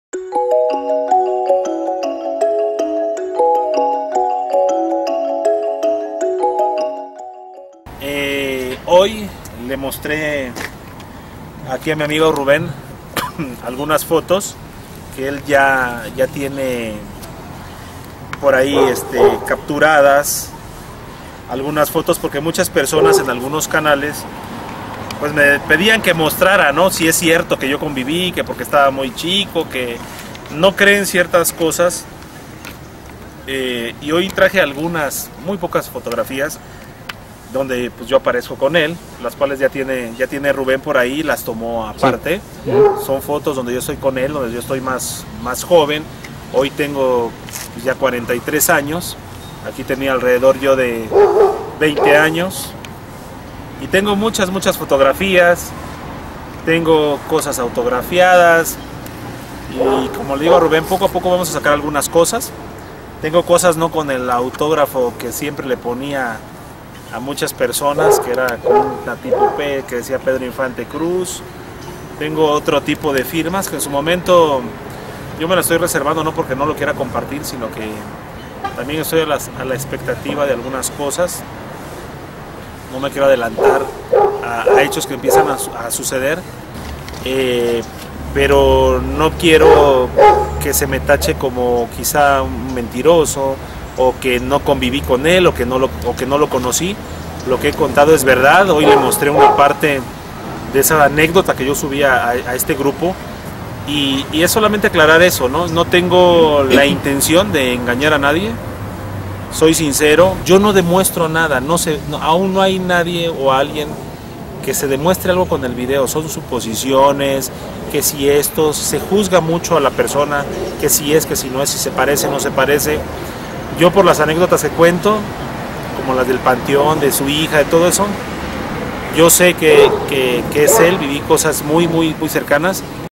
UNAS FOTOS CON ANTONIO P.INFANTE
El amigo Oscár, Omar muestra a todos nosotros algunas de las fotos que se tomo con el idolo de america latina,cuando el se presentaba cantandole a su público que era el 80% que el decia estaba con el.suscribete al canal,activa la campanita para recibir notificaciones de los nuevos videos.y comparte,es importante.